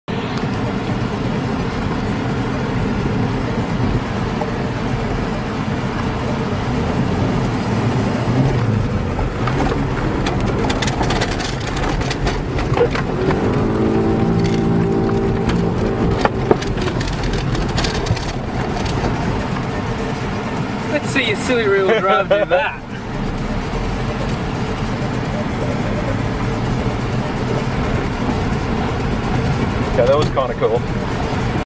dirt helis
After I made a comment about having "silly drive to the front wheels" Red cuts a few quick dirt helis in his Galant VR4